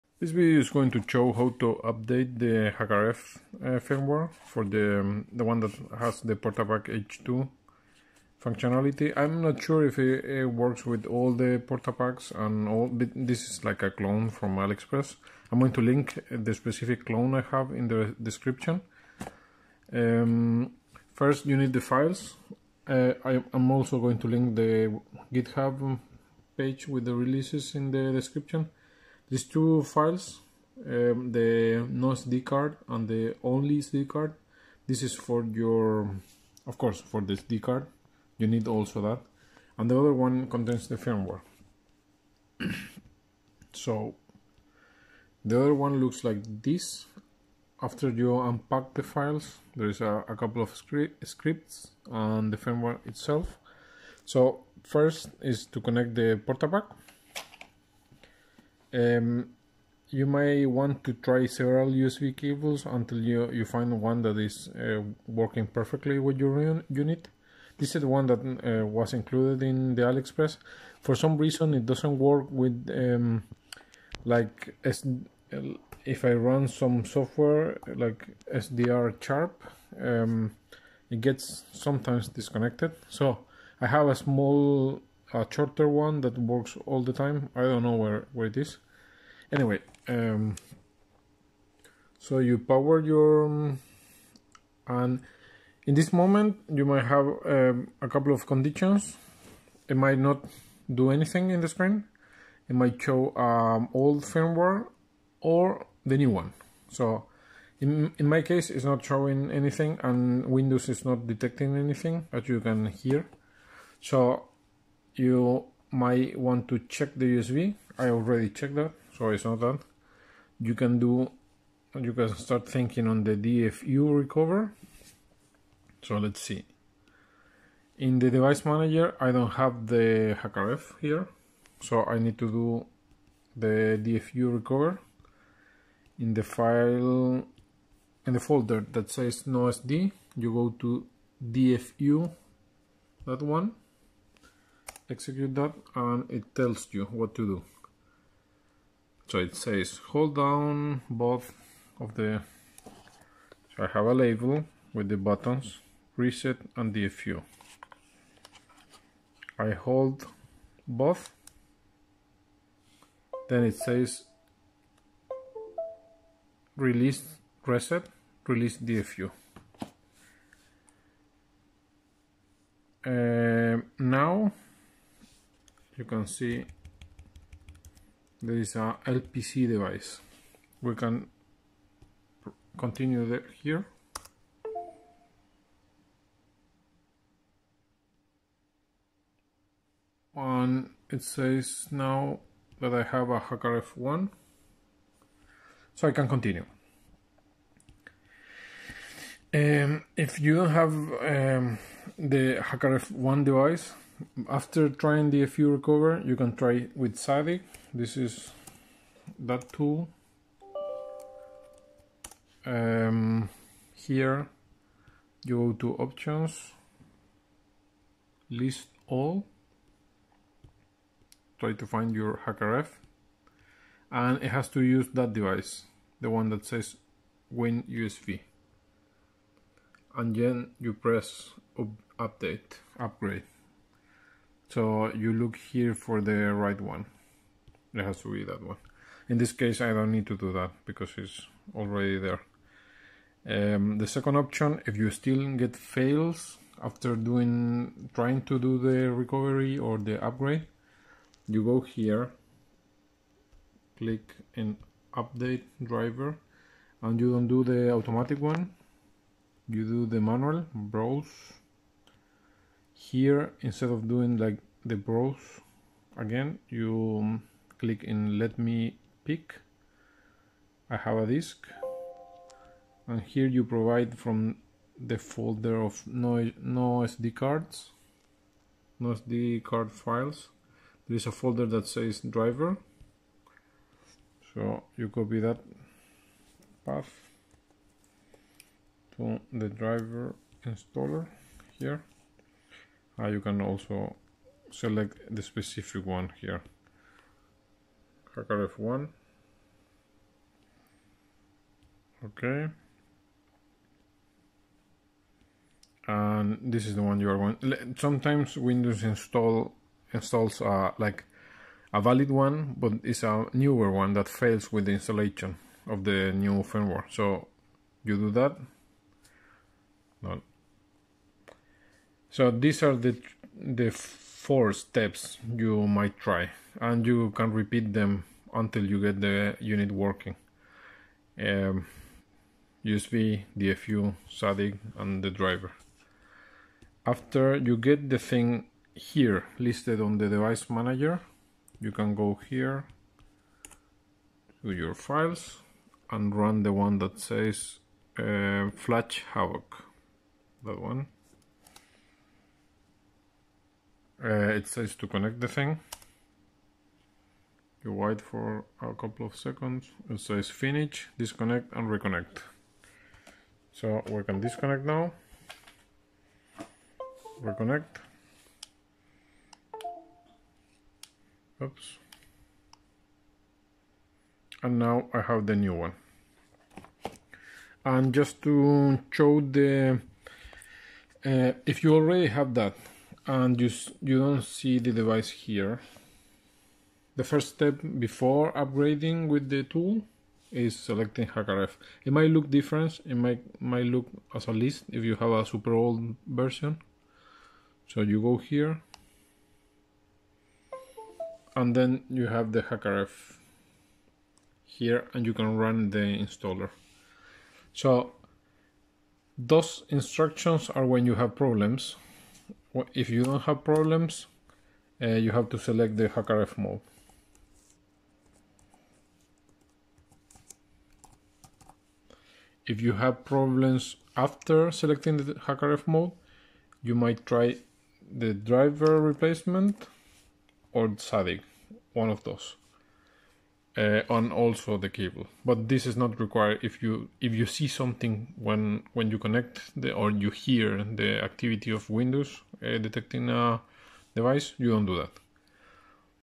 Portapack Havoc/Mayhem firmware upgrade
Upgrading the firmware may become a bit cumbersome. This video tries to explain all the problems and solutions.